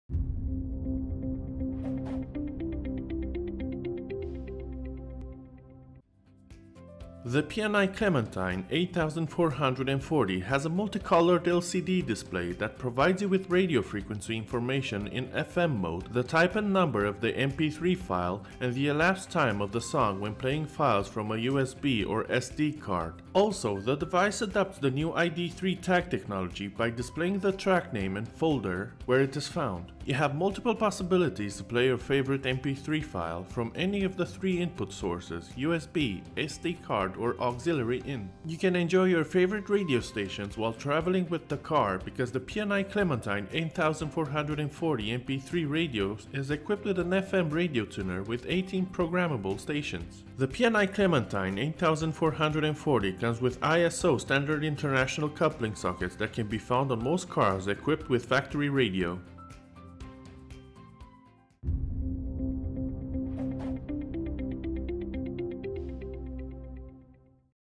Das PNI Clementine 8440 verfügt über ein mehrfarbiges LCD Display
Das PNI Clementine 8440 verfügt über ein mehrfarbiges LCD-Display, das Ihnen Radiofrequenzinformationen im FM-Modus, den Typ und die Nummer der MP3-Datei sowie die verstrichene Zeit des Songs beim Abspielen von Dateien von einer USB- oder SD-Karte liefert. Das Gerät übernimmt außerdem die neue ID3-Tag-Technologie, indem der Name und der Ordner des Tracks angezeigt werden, in dem er gefunden wurde.

Sie haben mehrere Möglichkeiten, Ihre bevorzugten MP3-Dateien von einer der drei Eingangsquellen abzuspielen: USB, SD-Karte oder Aux In.

Sie können Ihre Lieblingsradiosender mit dem Auto genießen, da das PNI Clementine 8440 Mp3-Radio mit einem UKW-Radio-Tuner mit 18 programmierbaren Sendern ausgestattet ist.

Der PNI Clementine 8440 ist mit internationalen Kupplungssteckdosen nach ISO-Standard ausgestattet, die bei den meisten Fahrzeugen mit Werksfunk vorhanden sind.